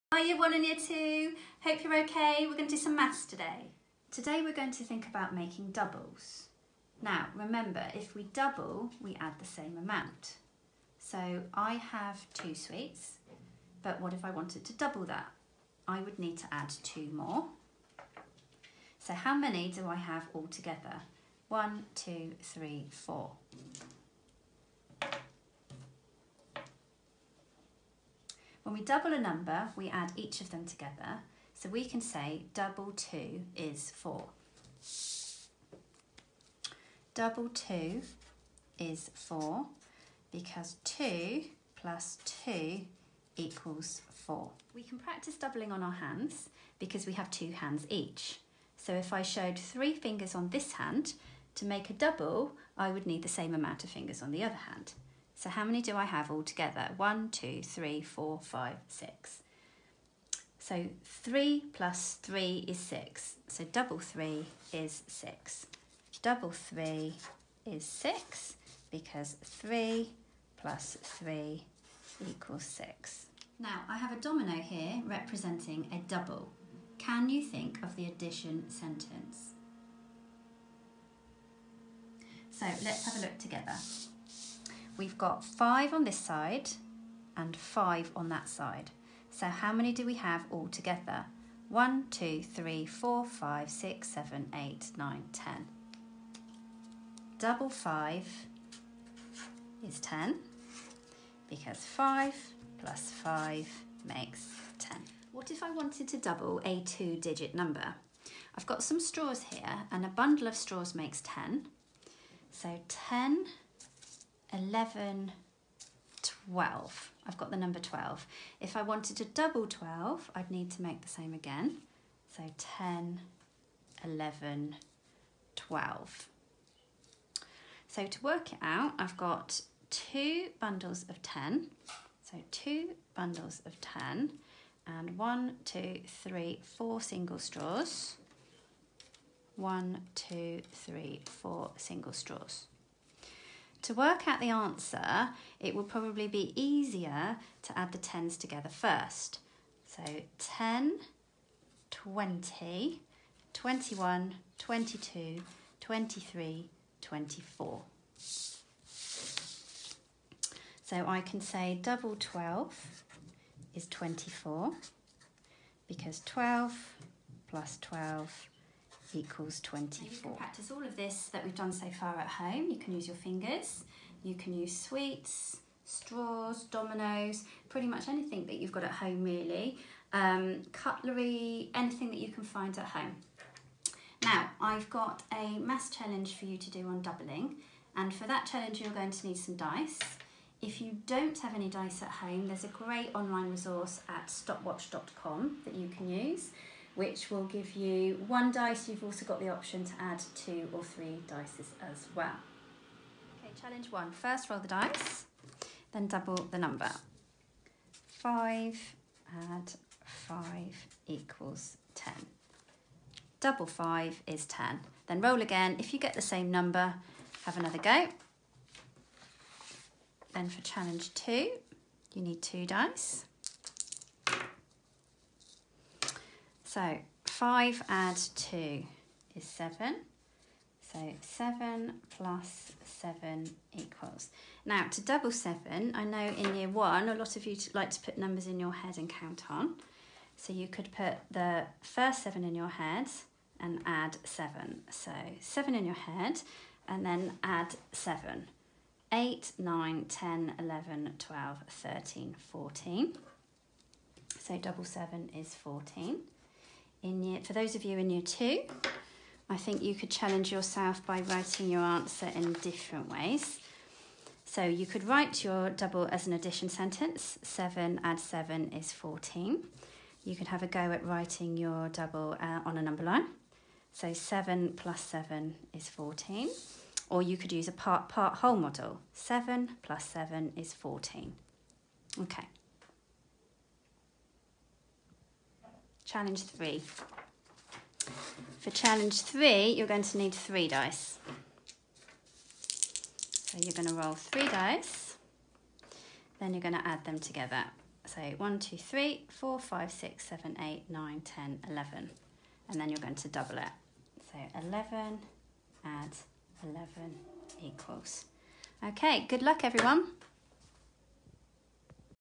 Miss Nichol’s Maths Challenge
Miss Nichol is ready with a maths challenge all about doubles.

Can you double a number?

Watch the video and have a go at some of Miss Nichol’s games.

Good luck.